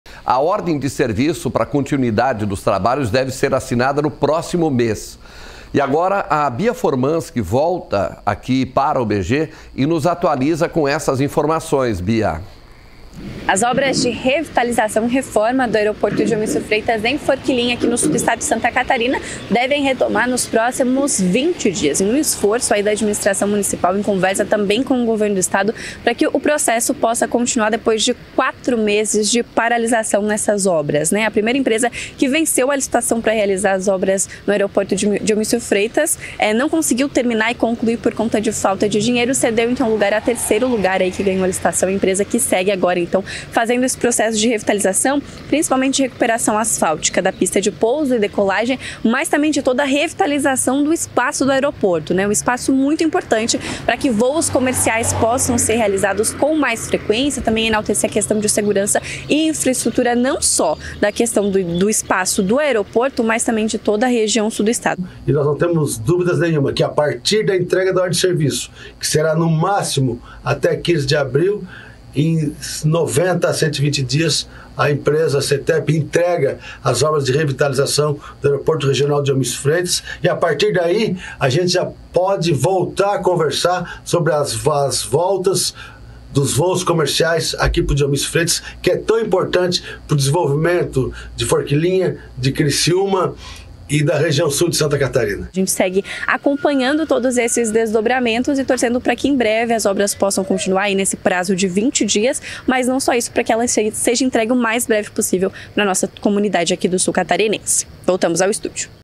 Obras no Aeroporto Diomício Freitas, de Forquilhinha, devem ser retomadas no próximo mês
As obras do Aeroporto Diomício Freitas, em Forquilhinha, estão paradas há quatro meses. Depois de uma reunião em Florianópolis, o prefeito da cidade, José Cláudio Gonçalves, o Neguinho, destaca boas notícias: os trabalhos devem ser retomados no próximo mês.